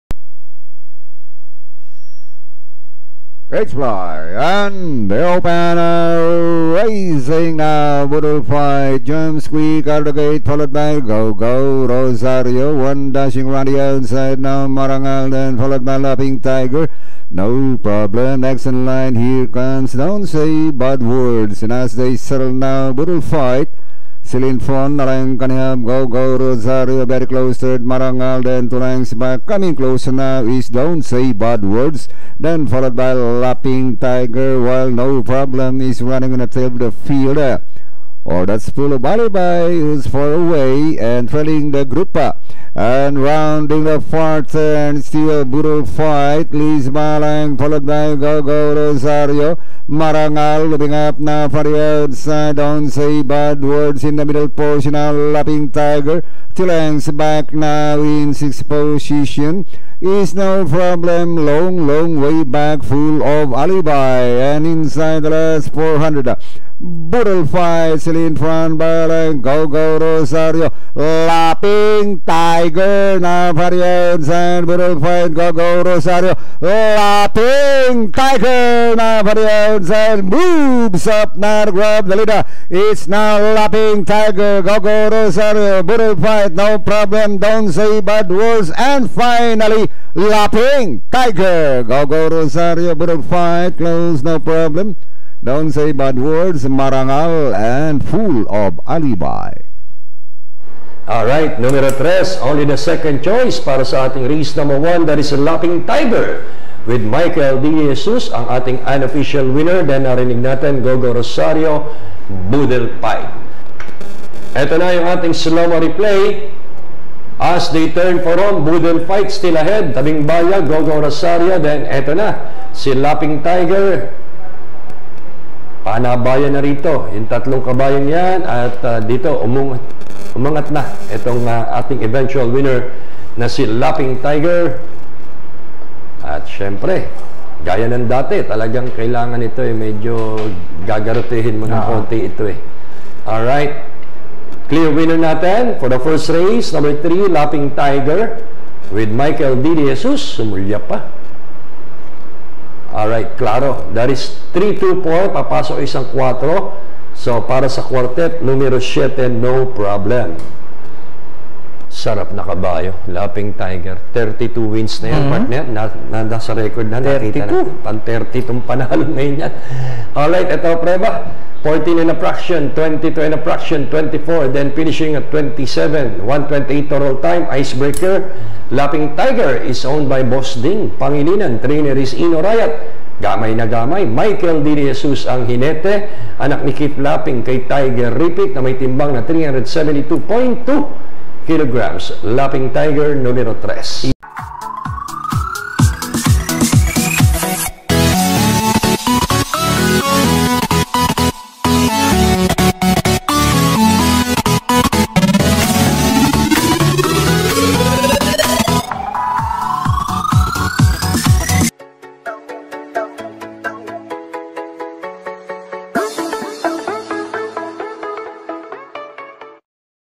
LAUGHING TIGER - MMTCI RACE 1 | JANUARY 7, 2022 | Metro Manila Turf Club BAYANGKARERISTA REPLAY
BAYANGKARERISTA RACE REPLAY at METRO MANILA TURF CLUB
LAUGHING TIGER - JANUARY 07, 2022 RACE 7 - MMTCI METRO TURF LIVE RACING